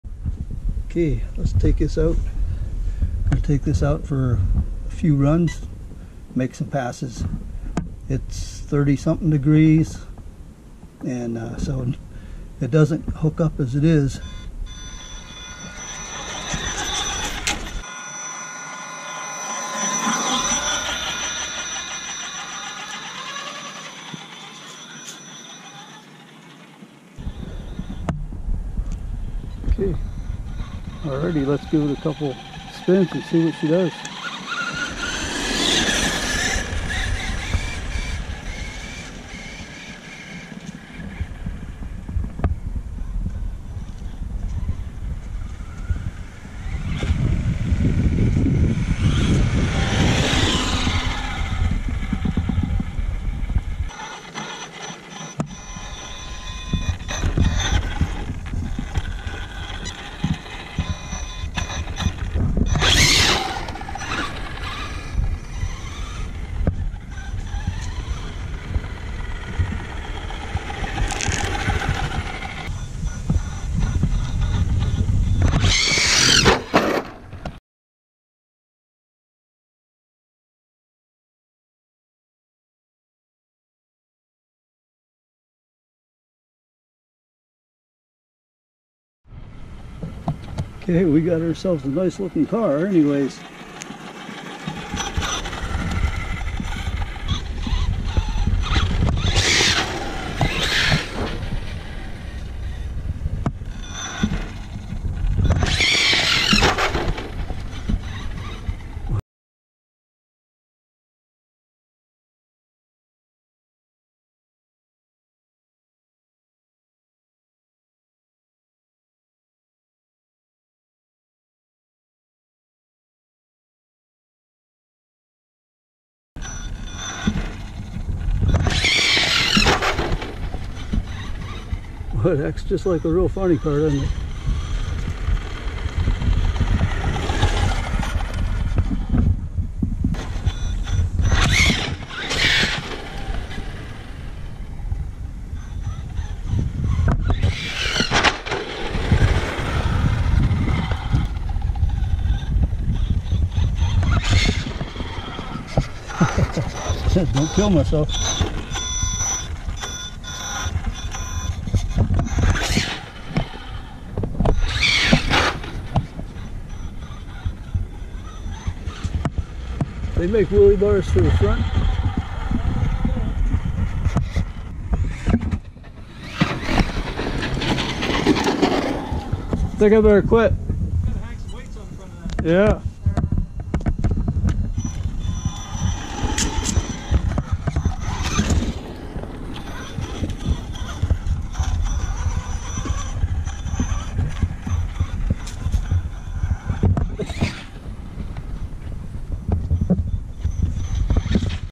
Test hits
Trying out the Camaro on 3s. Random gears